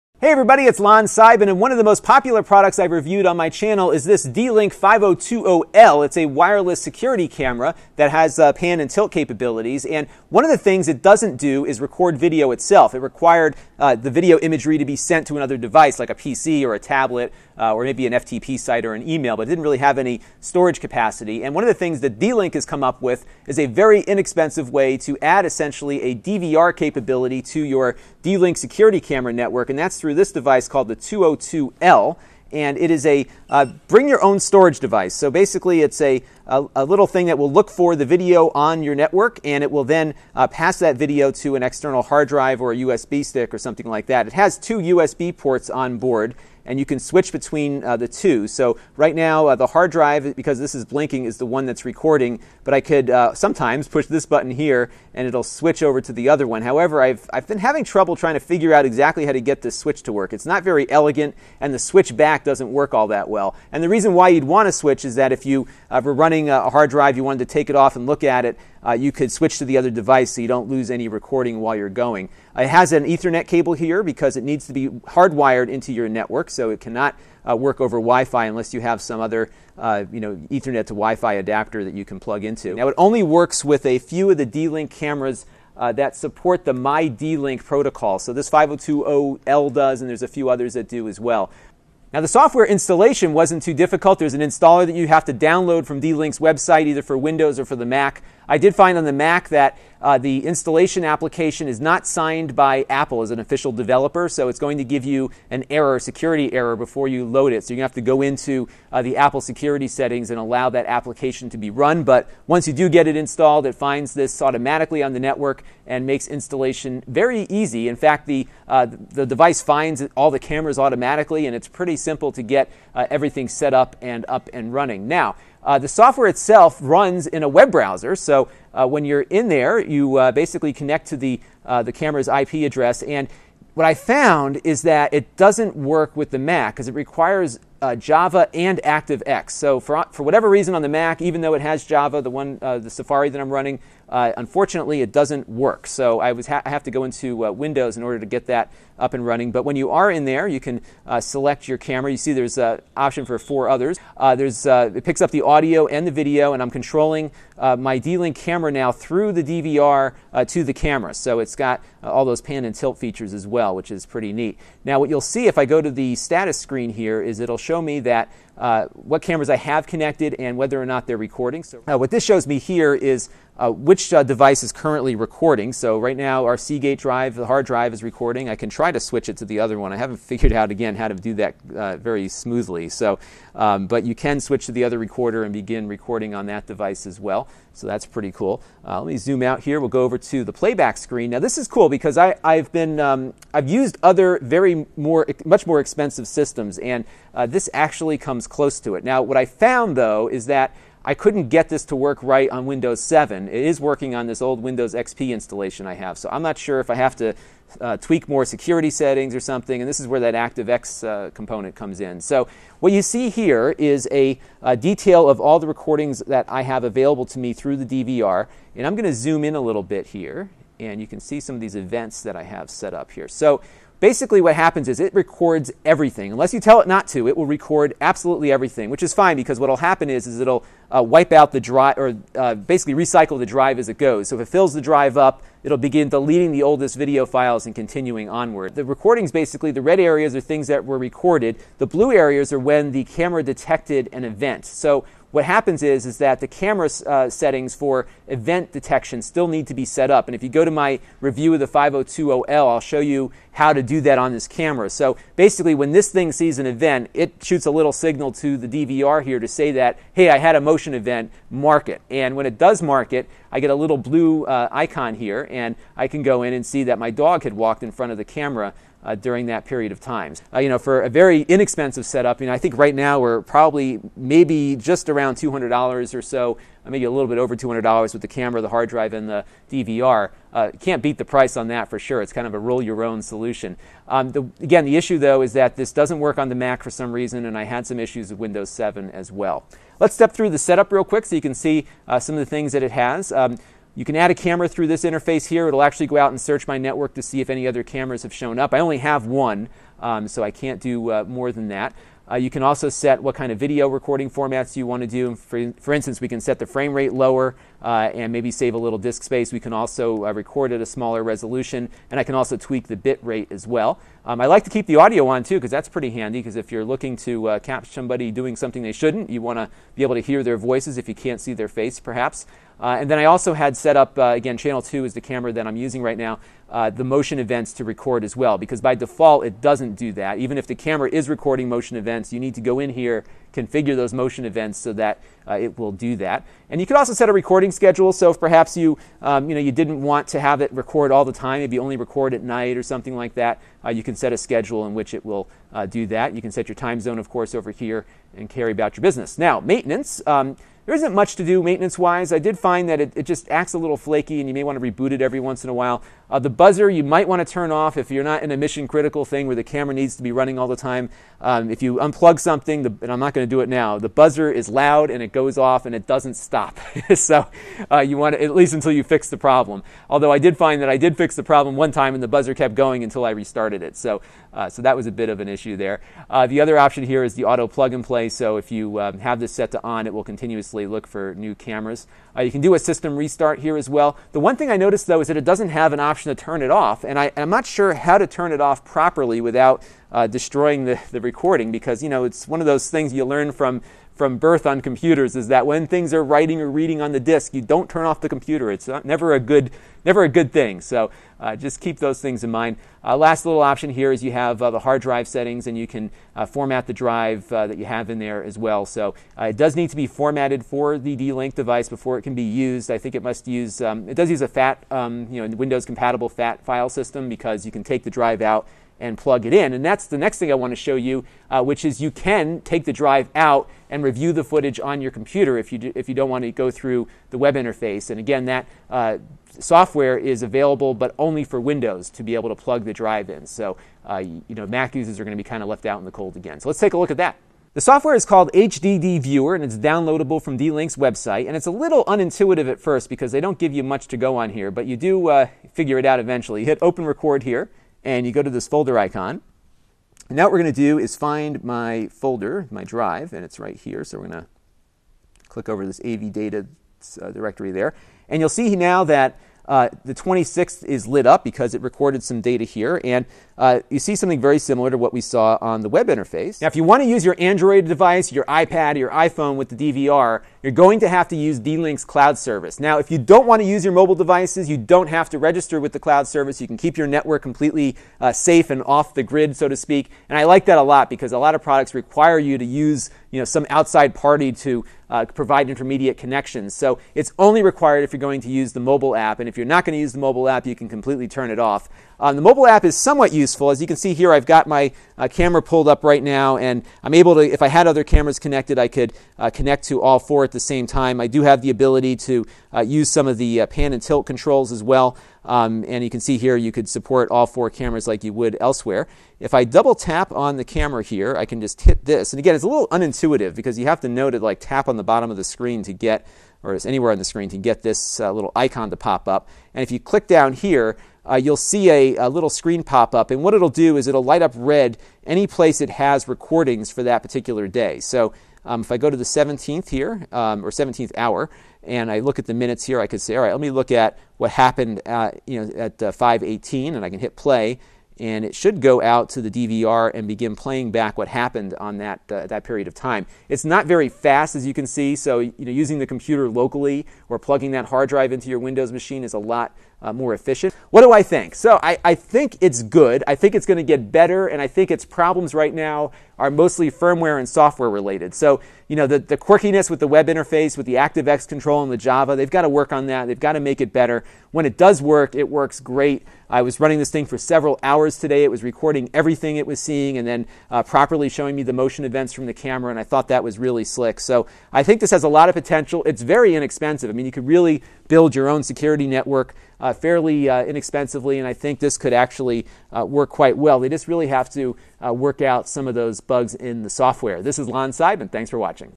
D Link Camera Video Recorder mydlink Enabled
D Link Camera Video Recorder having been a mydlink camera user for over 3 years now, and growing my home to 4 cameras, this device fits in perfectly to my video surveillance arsenal. Setup was a total breeze and it connected to all my cameras immediately. Being able to view all 4 cameras on the iPhone NVR app is worth the price alone. Still working on configuring the external hard drives and the recording events and schedules, and will amend the review at that time. For now, for the price paid, this product is well worth it!